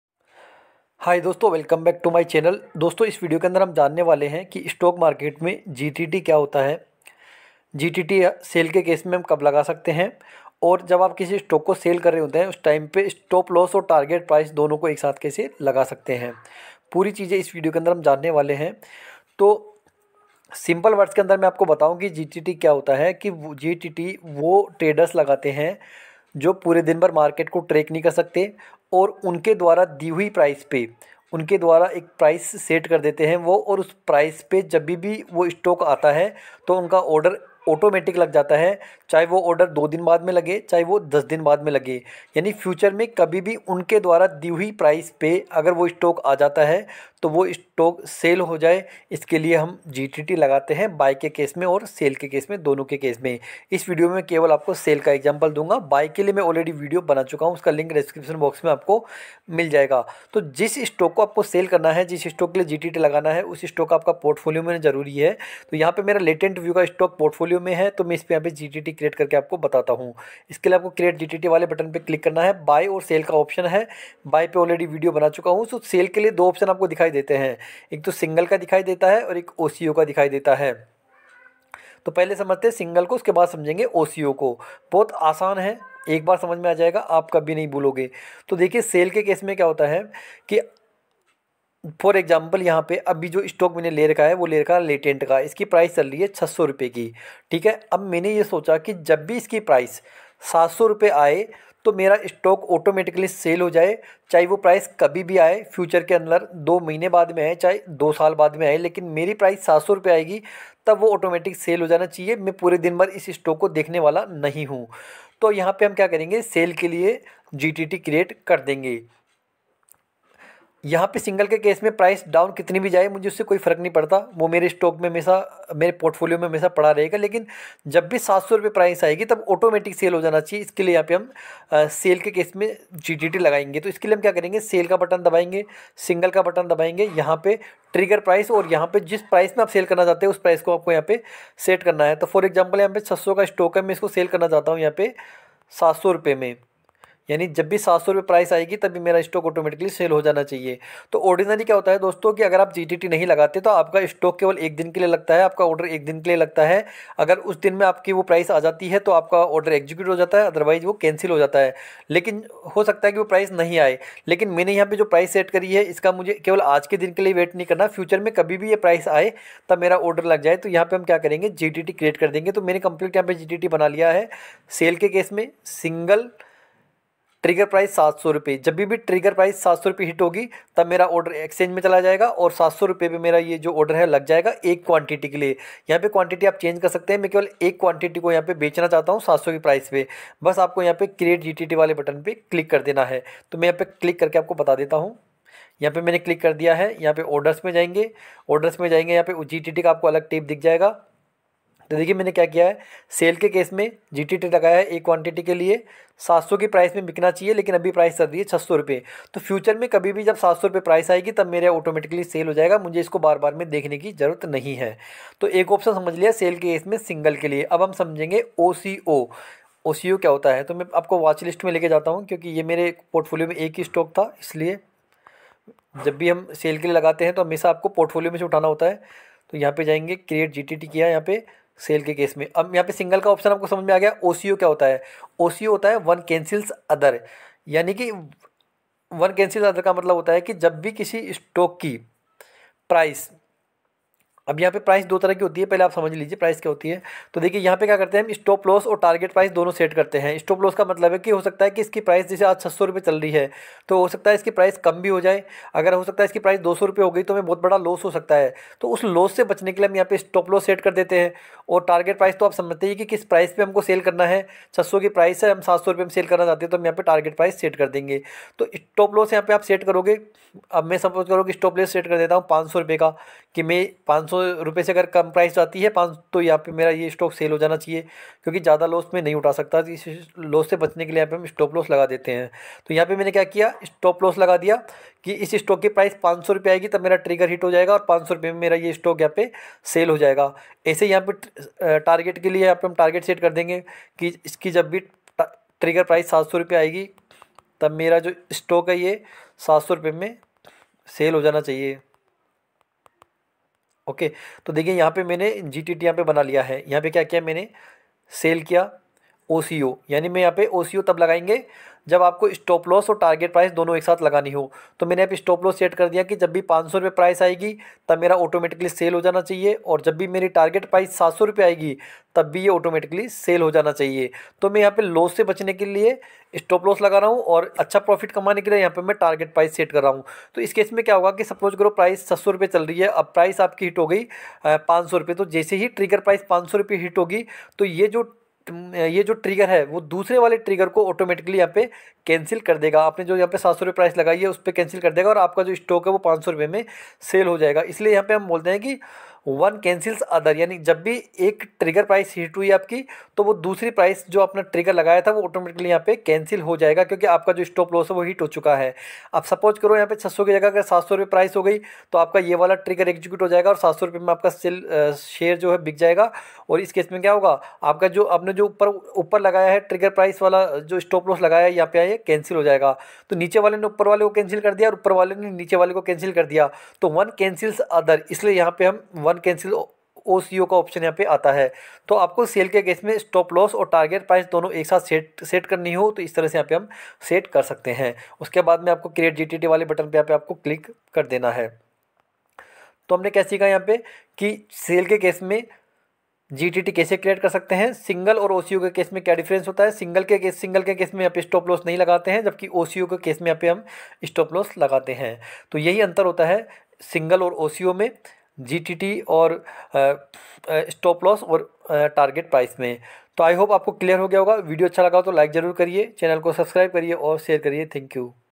Single vs oco in zerodha | What is GTT | trigger type oco in zerodha | stoploss and target zerodha
Hi Guys

In the video below topics covered:
What is GTT in stock market
Trigger type Single Vs OCO
How to put stoploss and Target in Gtt Sell Order
We will place a Gtt Sell Single order live in the video.

stop loss gtt zerodha,
stoploss in gtt zerodha ,zerodha me gtt stop loss kaise lagaye ,gtt stop loss angel broking ,